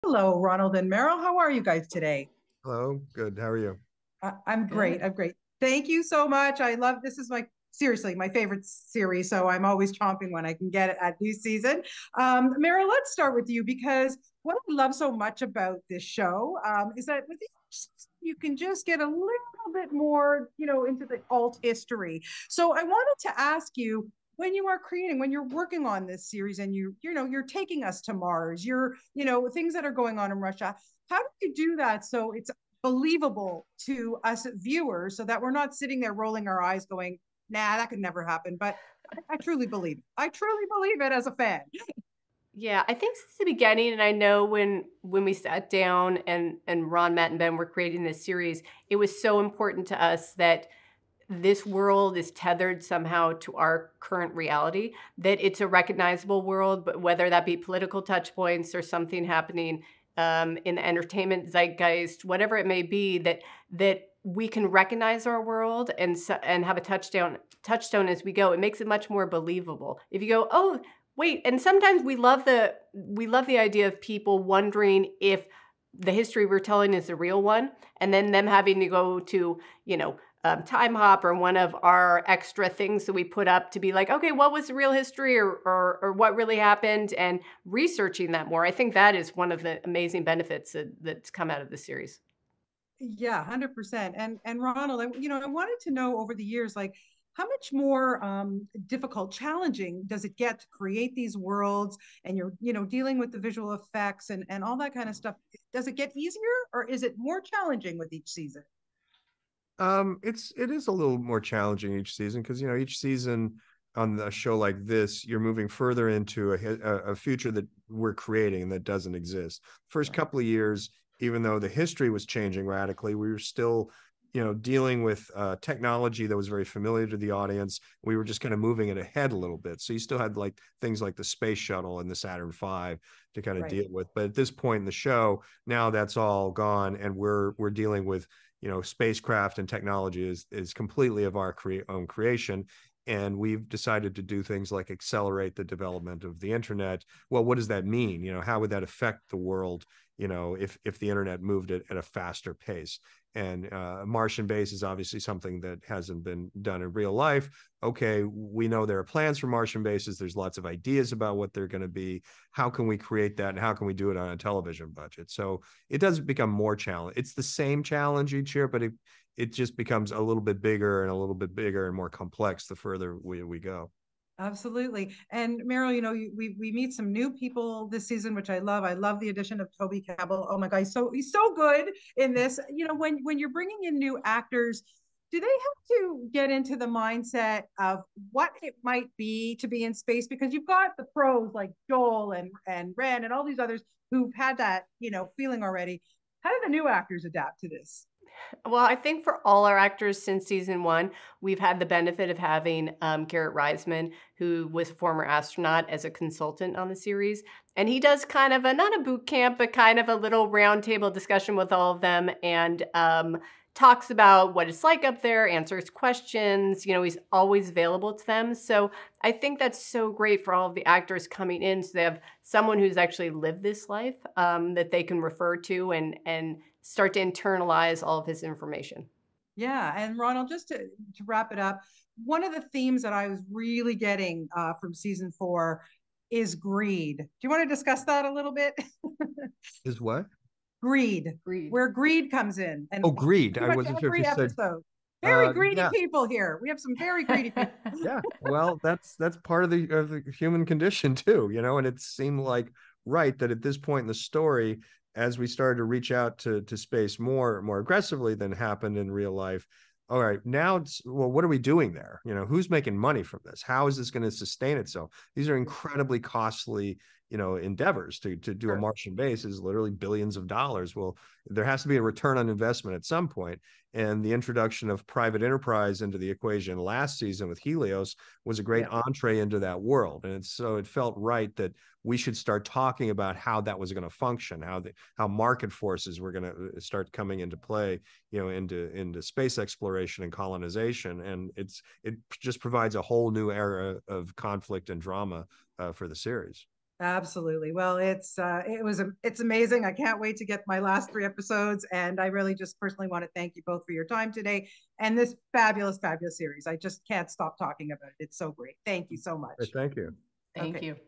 FOR ALL MANKIND (S4) RONALD D. MOORE & MARIL DAVIS INTERVIEW (2023)
Bonnie Laufer @movi_boni talks to RONALD D. MOORE & MARIL DAVIS about Season 4 of their hit Apple TV+ series.  For All Mankind